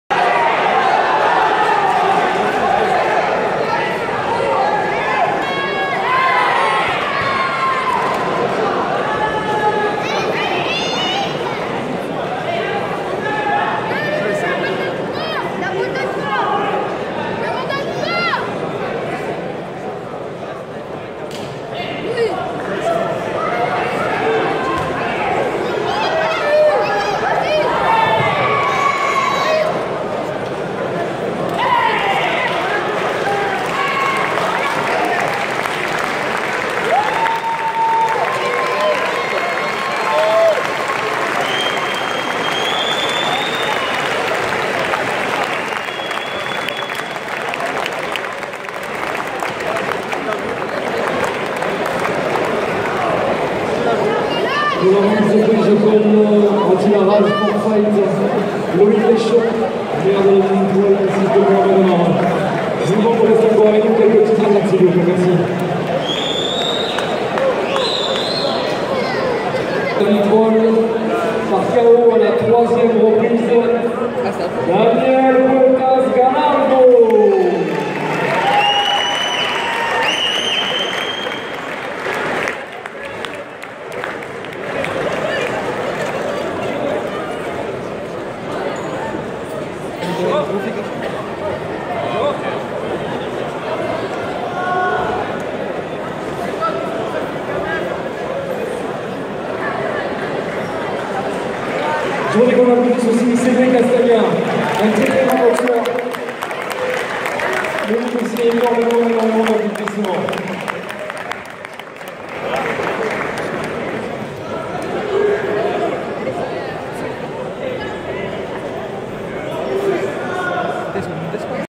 Championnat du monde K1 France vs Espagne 65Kg 24 Février 2018 Salle Maurice Piot Team Amara
Stars Night Vitrolles du 24 Février 2018 Salle Maurice Piot
Cédric Castagna (FR) vs Daniel Puertas (ESP) Championnat du monde de K1 ISKA 65kg (5x3')